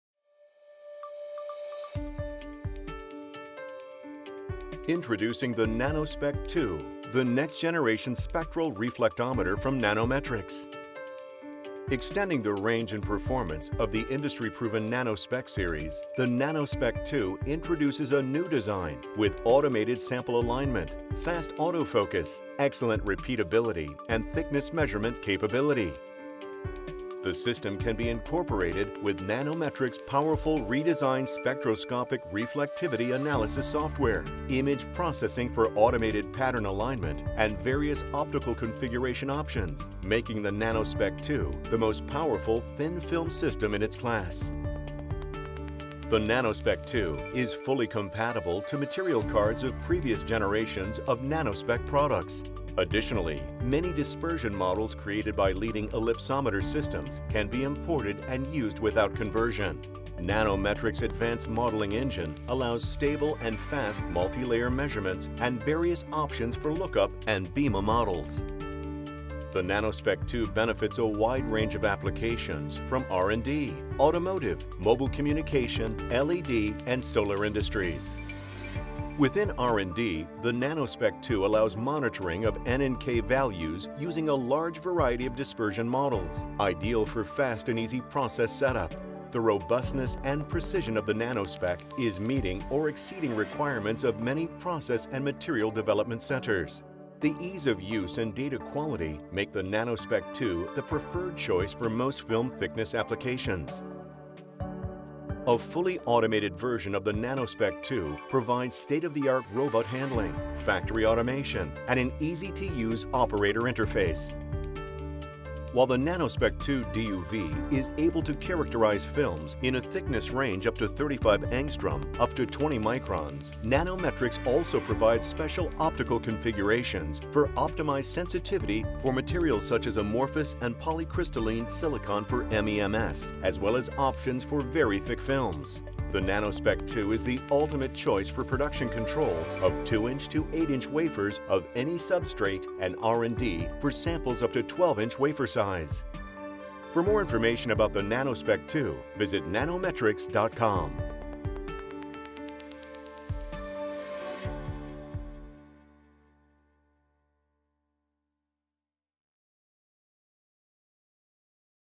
Advanced Film Analysis with the NanoSpec II
The Advanced Film Analysis System can be incorporated with Nanometrics' spectroscopic reflectivity analysis software, image processing for automated pattern alignment and various optical configuration options, making the NanoSpec II automated the most powerful thin film system in its class.